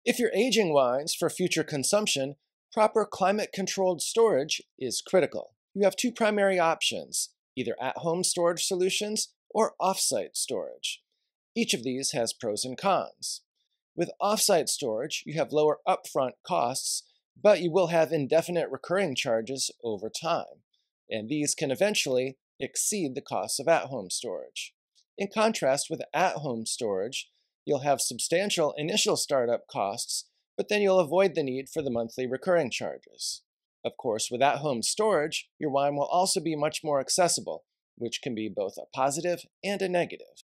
Wine Collecting Tip No. 6: WINE STORAGE 🍷
John Jackson, DipWSET a/k/a Attorney Somm provides a wine collecting tip relating to wine storage. Specifically, Attorney Somm introduces the two main storage options for wine collectors and some pros and cons of each wine storage solution.